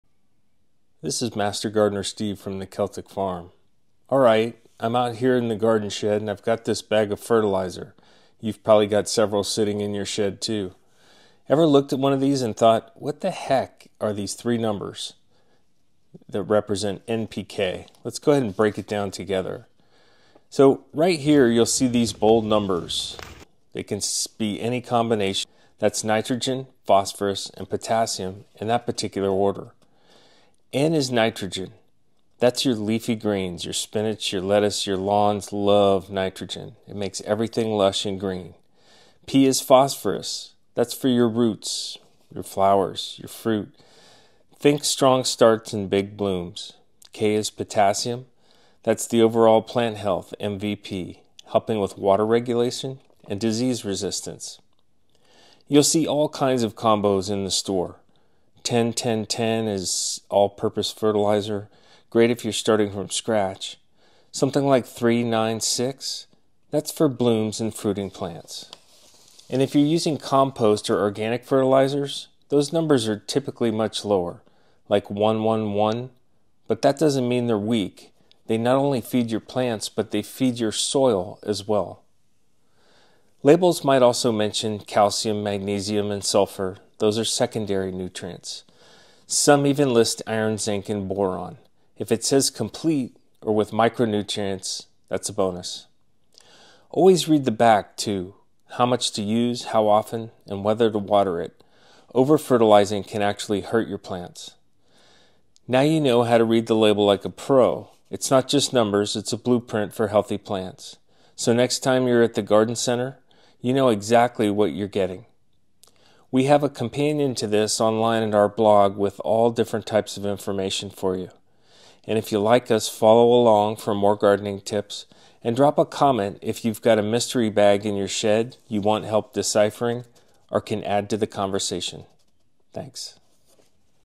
Are You Using Fertilizer Wrong? What N-P-K Really Means
Think all fertilizers are the same? Think again. Those mysterious numbers on the bag—N-P-K—could be the reason your garden isn’t thriving.

In this video, I break down exactly what N-P-K means, how to choose the right fertilizer for your plants, and what most gardeners get totally wrong.

🎥 Shot from my POV using Meta Glasses—so you’re seeing what I see in the garden.

👩‍🌾 Stop guessing. Start growing smarter.